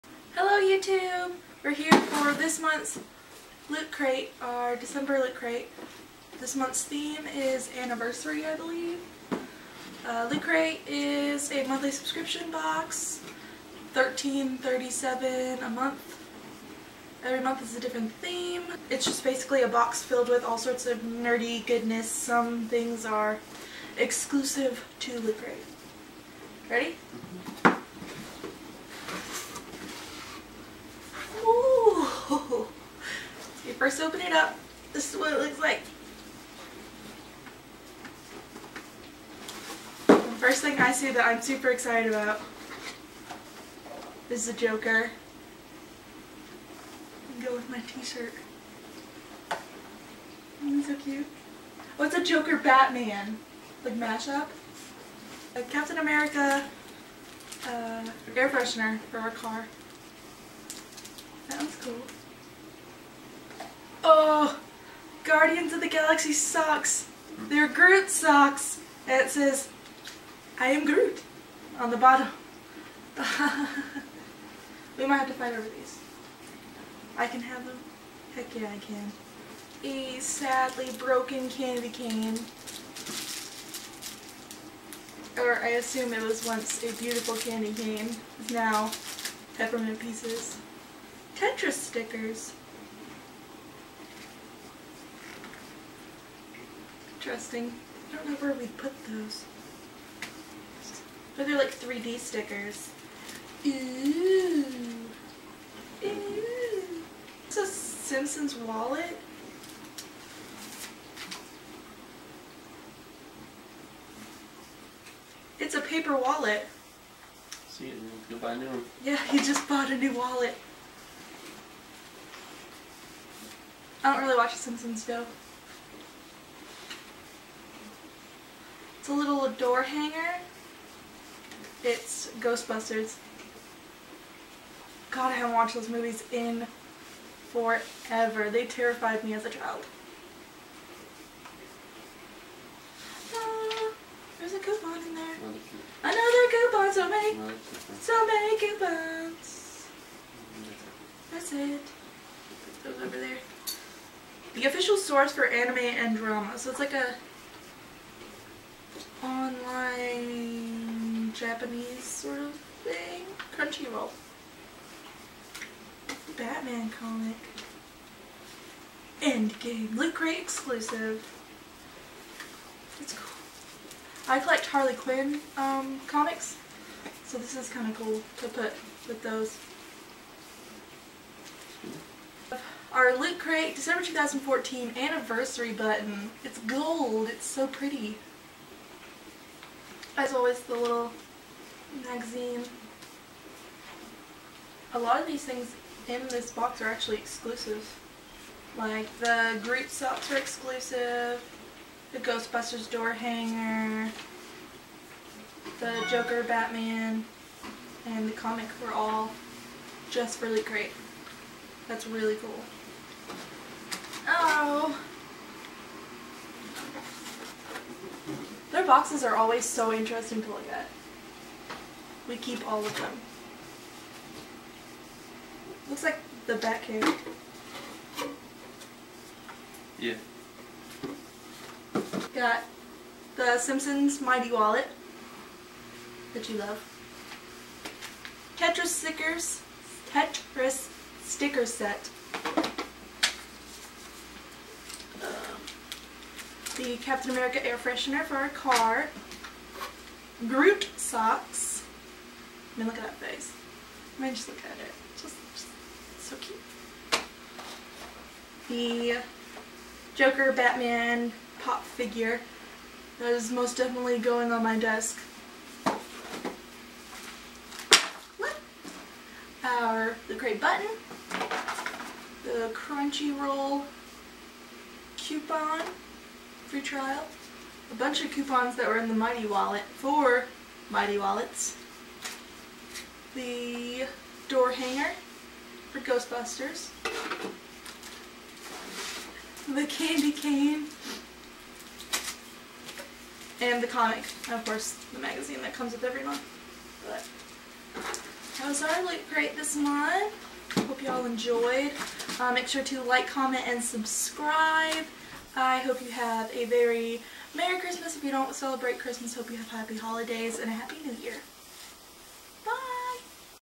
December Loot Crate Unboxing: Anniversary!
James, Max and I open our December Loot Crate! I really loved this box. So Batman, Much Love! Also, James was exhausted all day, bless him. Big thanks to the hubs for enduring this video :P